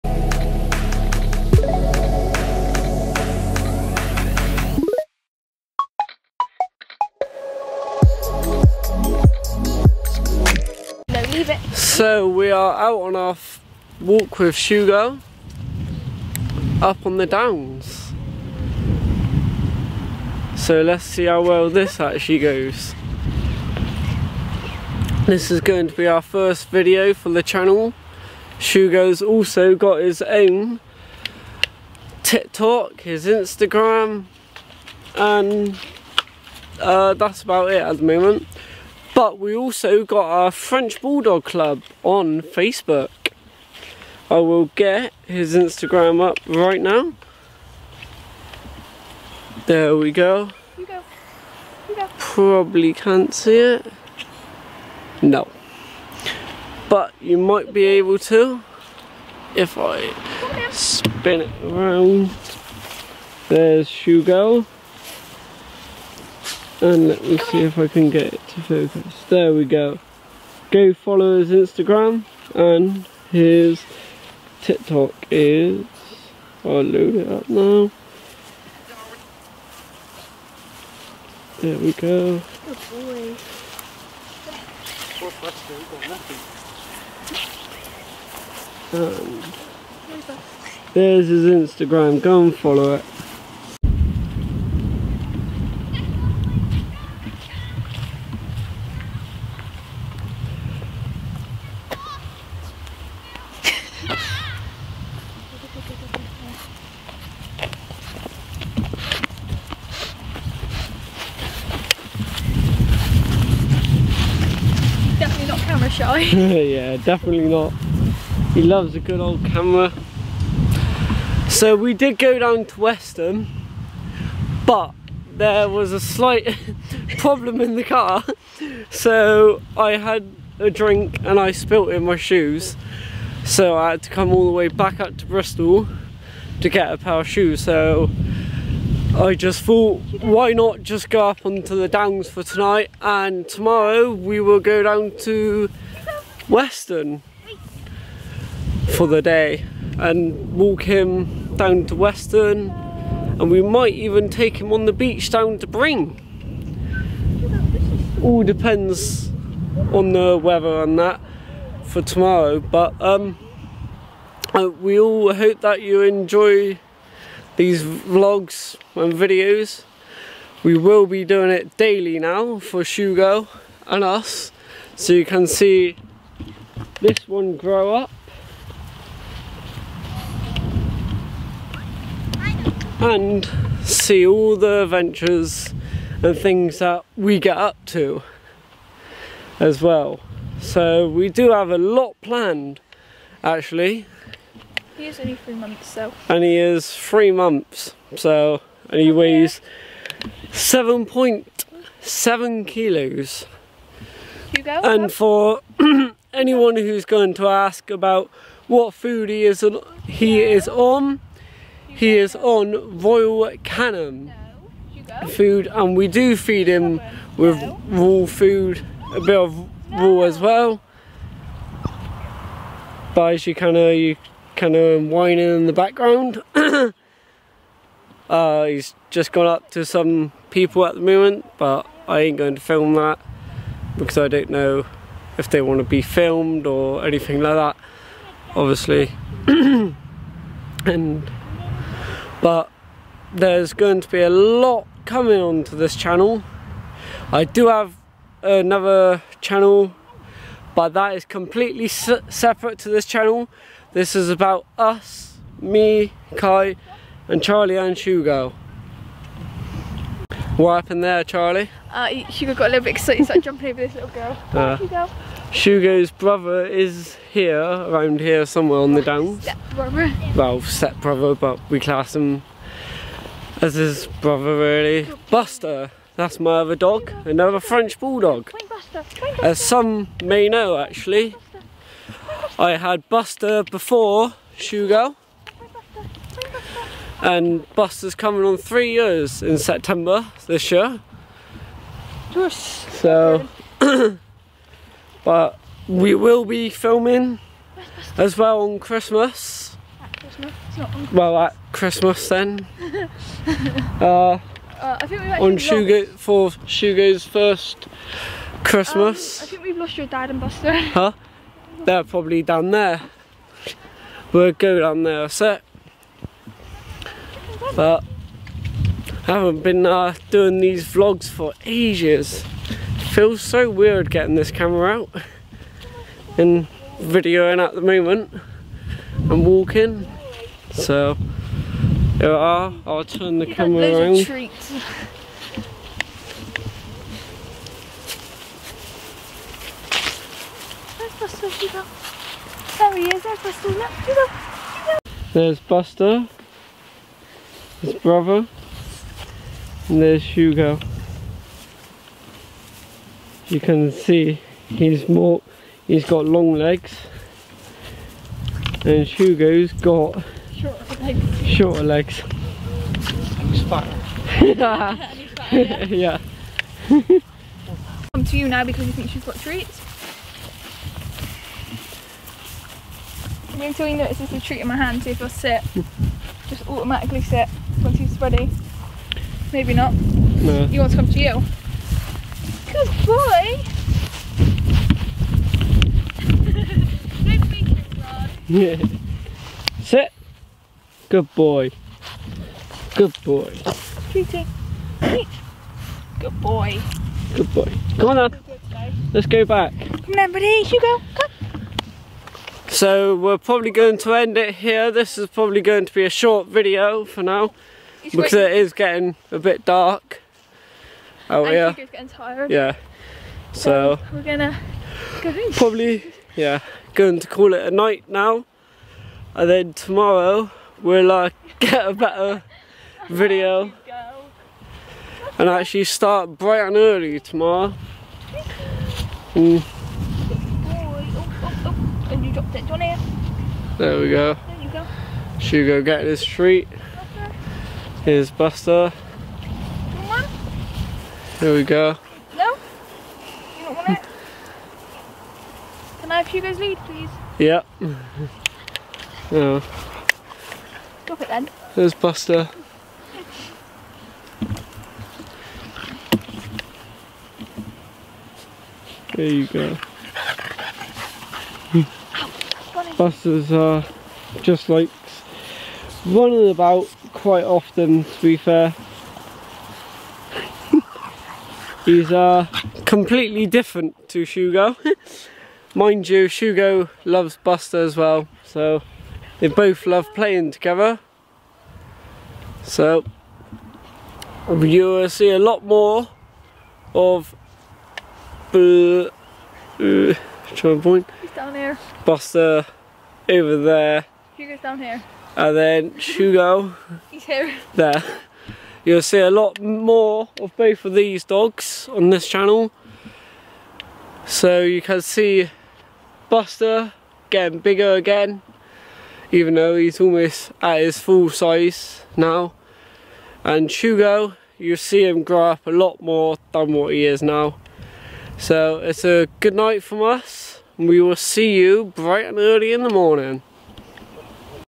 Vlog 1-First video
Sunday 24th July 2022, walk at Clifton downs